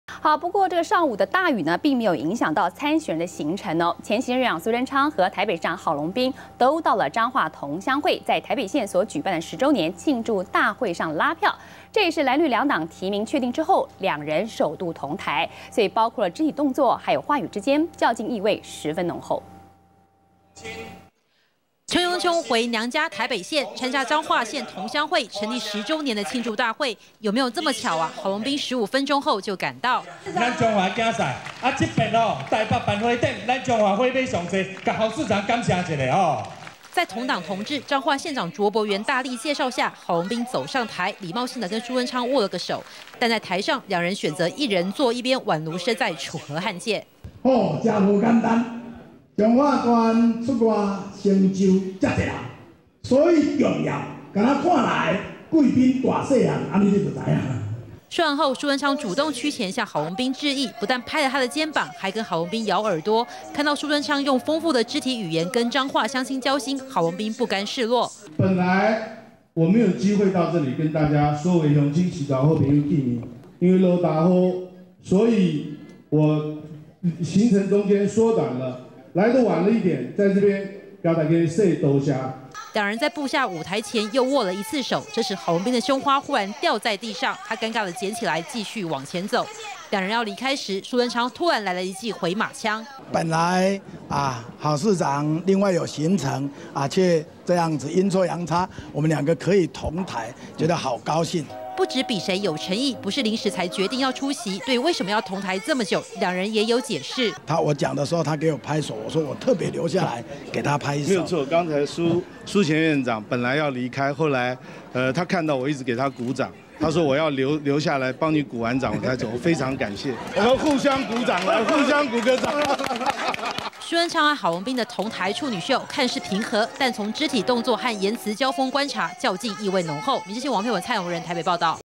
藍綠提名確定後 郝蘇首度同台－民視新聞
【民視即時新聞】前行政院長蘇貞昌，和台北市長郝龍斌，上午都到彰化同鄉會，在台北縣舉辦的10週年慶祝大會拉票，這也是藍綠兩黨提名確定後，兩人首度同台，也把台北市的戰火，延燒到新北市來。